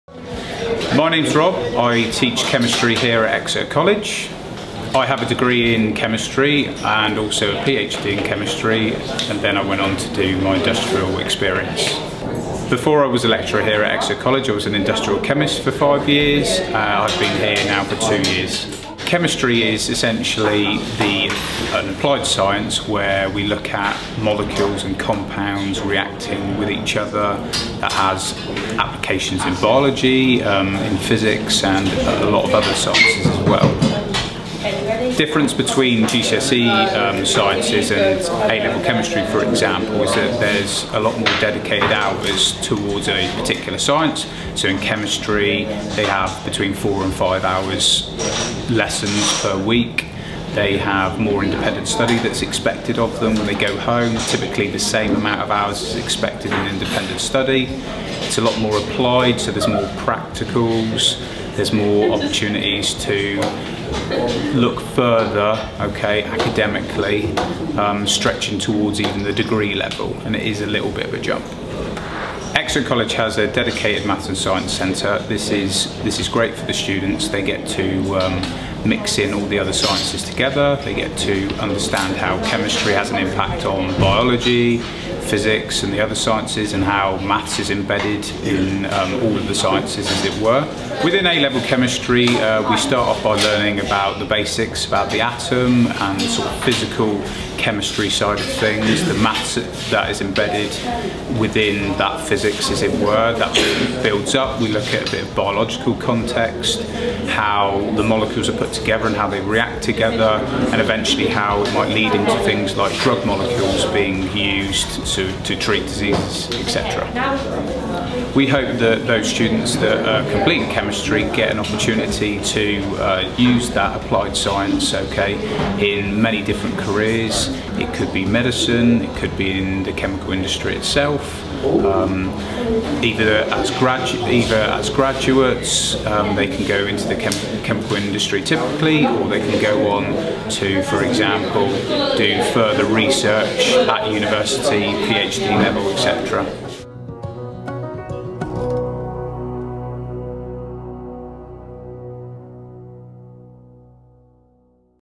Thinking of studying Chemistry at Exeter College?
What's it like to do Chemistry at A Level? Rob, a Chemistry lecturer, from Exeter College explains what chemistry is, what's involved in studying it at college and what we hope you'll go on to do.